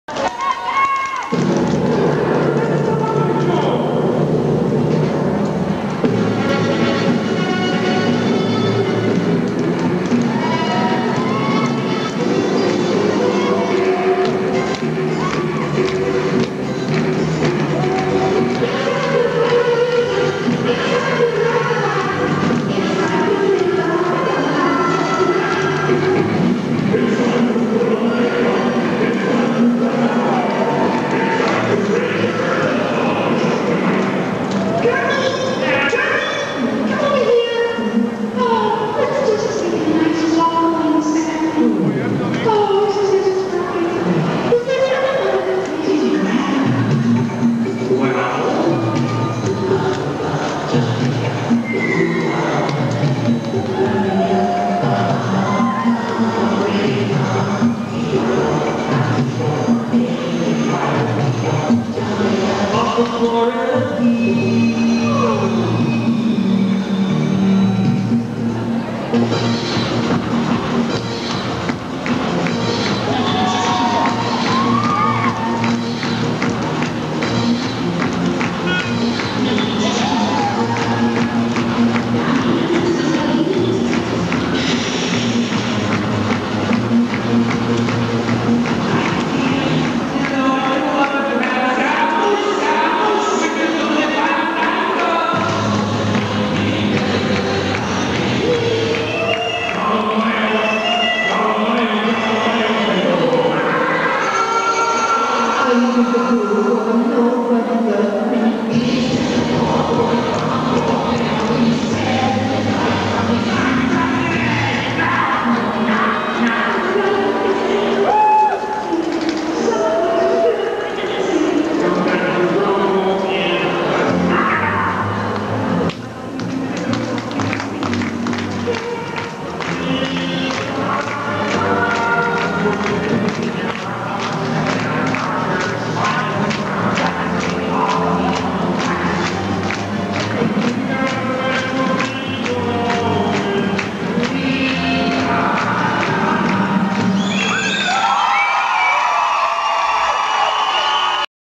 Rebecca and Shelley Hooper
England Junior Duo Accessories
NBTA European Majorettes Championships
Neuchatel Switzerland 2010